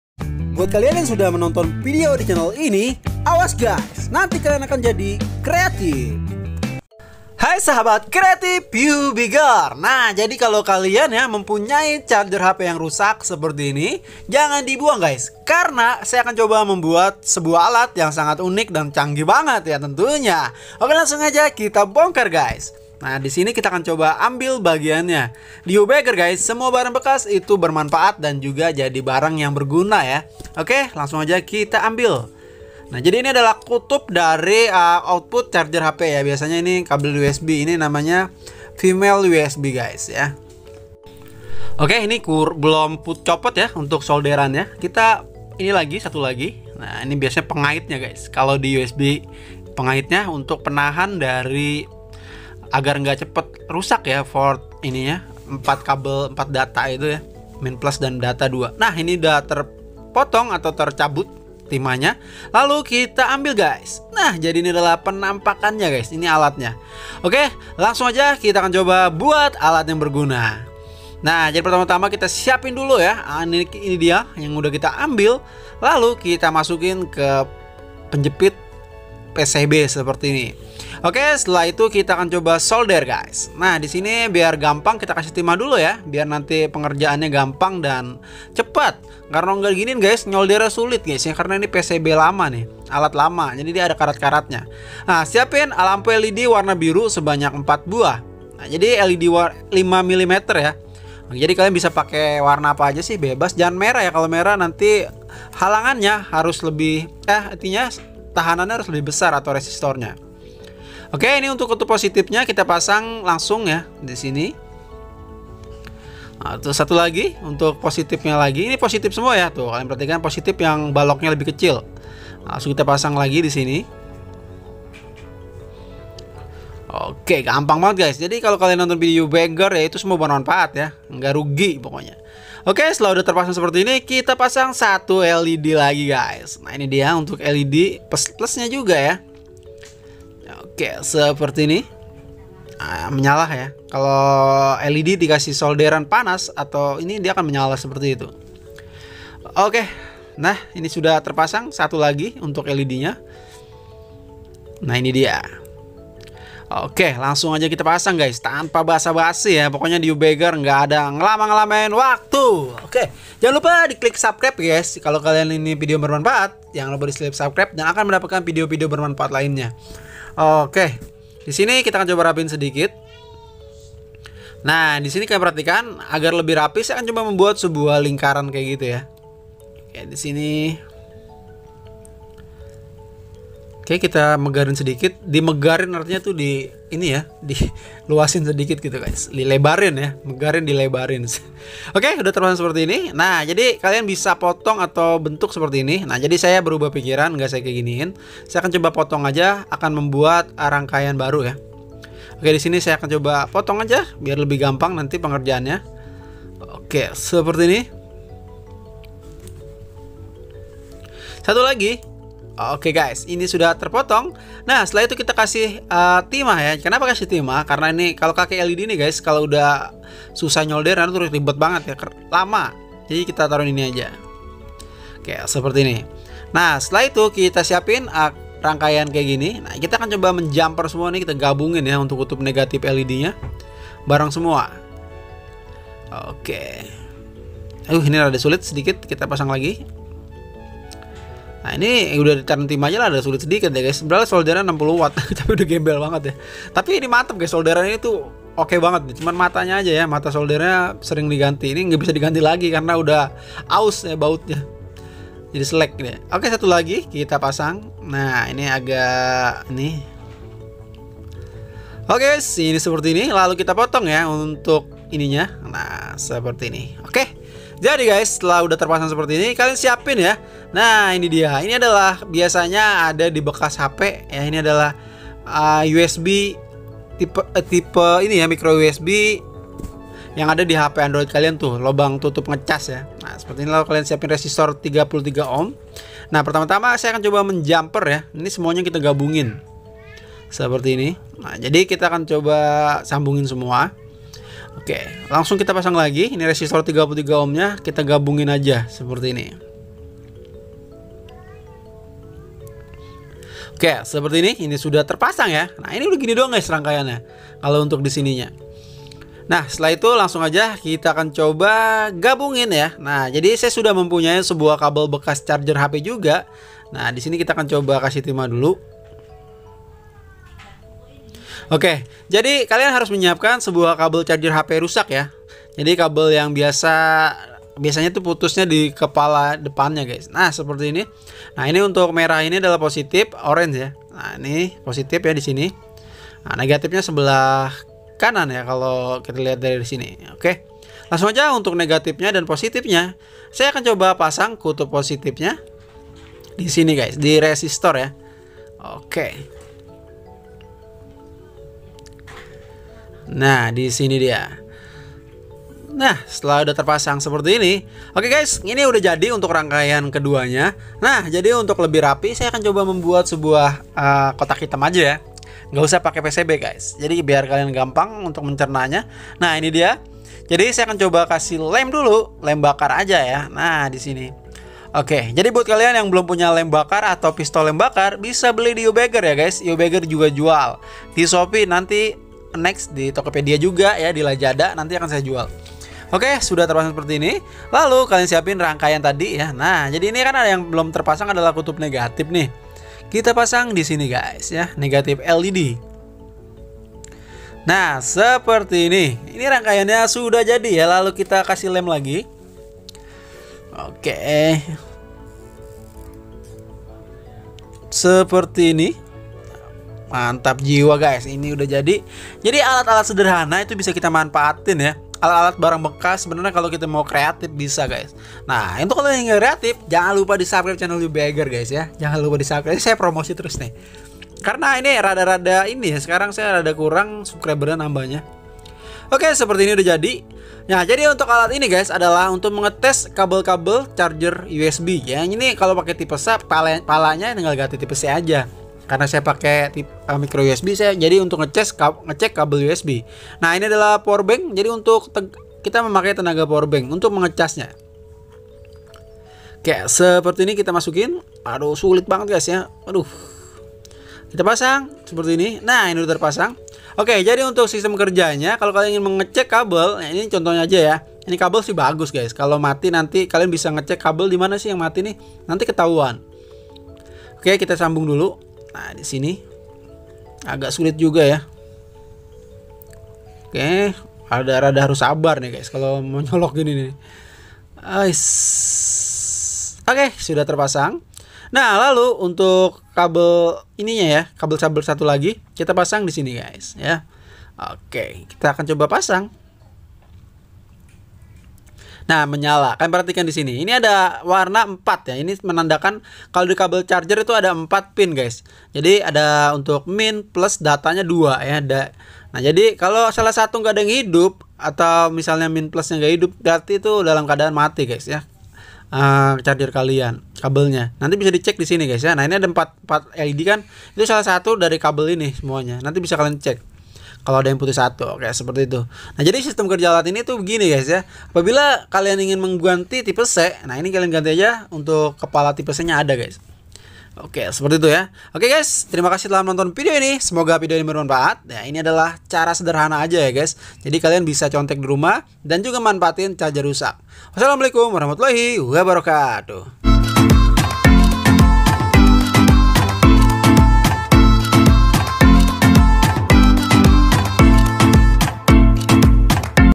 MODAL RP.0 !!! cara buat alat pengetes kabel Charger USB
hai sahabat kreatif youbeagger, saya mempunyai sebuah charger hp rusak. saya punya ide kreatif yaitu membuat sebuah alat cek Kabel USB dari CAS hp rusak. alat ini sangat berguna banget untuk mengetes kabel putus.

Jika ada pertanyaan , ajukan pertanyaan kalian di komentar !